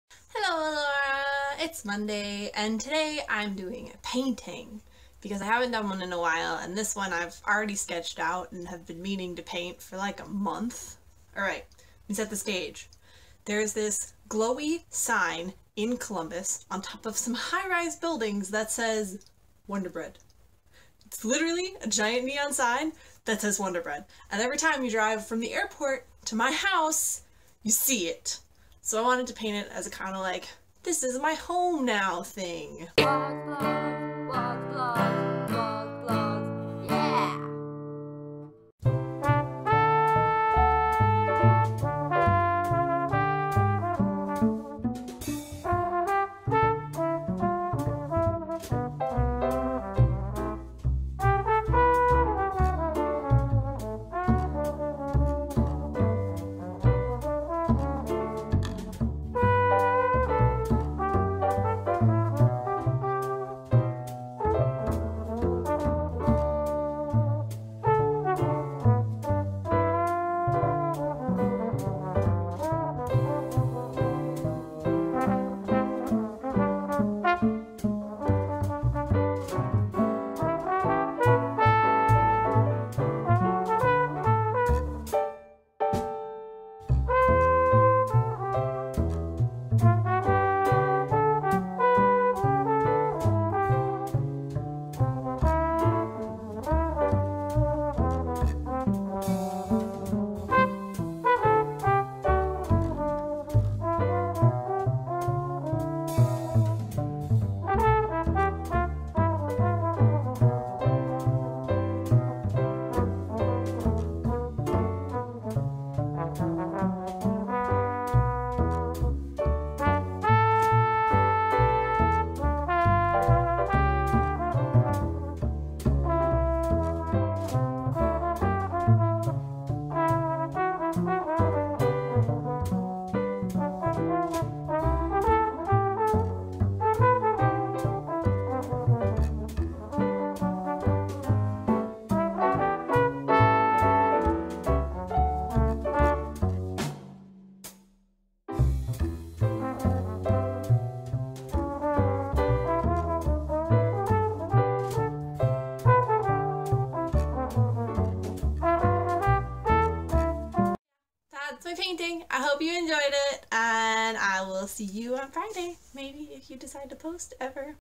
Neon Wonderbread Watercolor | Brittan [CC]
Why yes this neon sign does exist IRL in Columbus, OH.